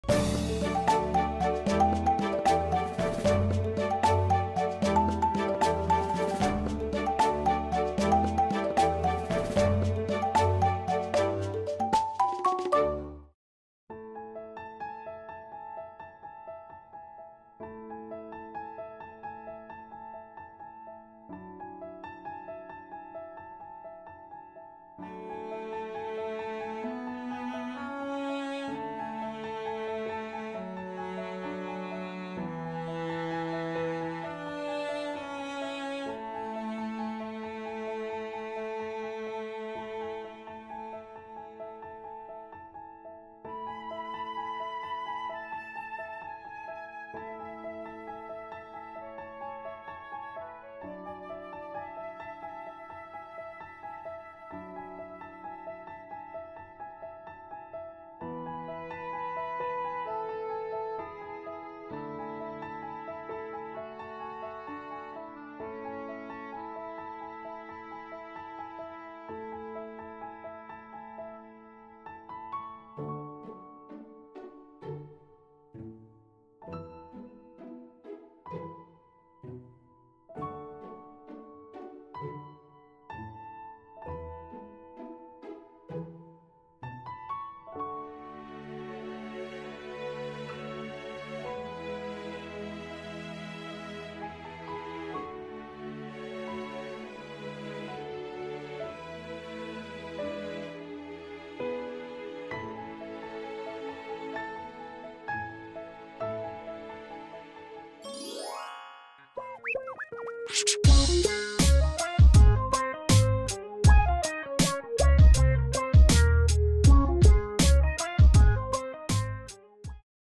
【大型犬の日常】とにかく甘えっ子なバーニーズマウンテンドッグ 【Bernese Mountain Dog】
毎晩、バーニーズマウンテンドッグ 2歳の恵美さんは甘えっ子です💕